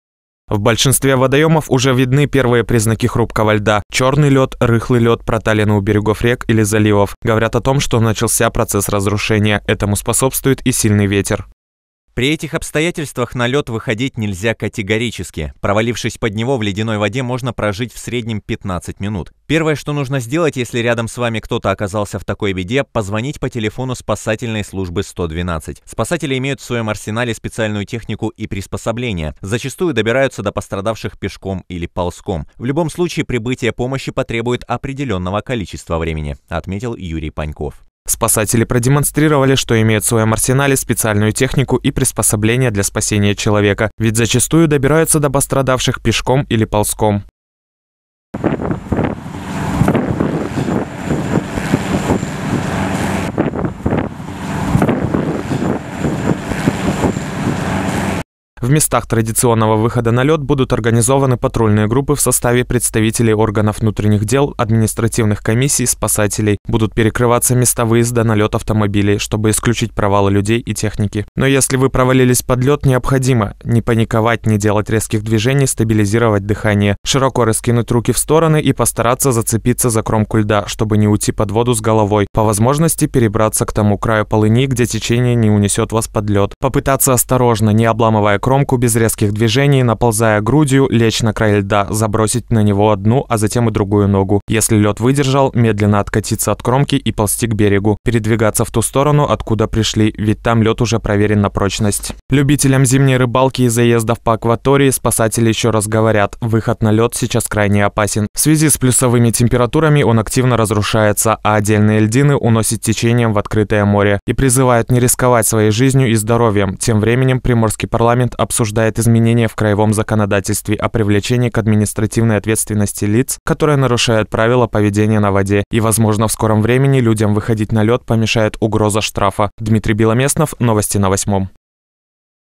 Лёд опасен
В большинстве водоемов уже видны первые признаки хрупкого льда: “черный” лед, рыхлый лед, проталины у берегов рек или заливов – говорят о том, что начался процесс разрушения льда. Этому способствует и сильный ветер.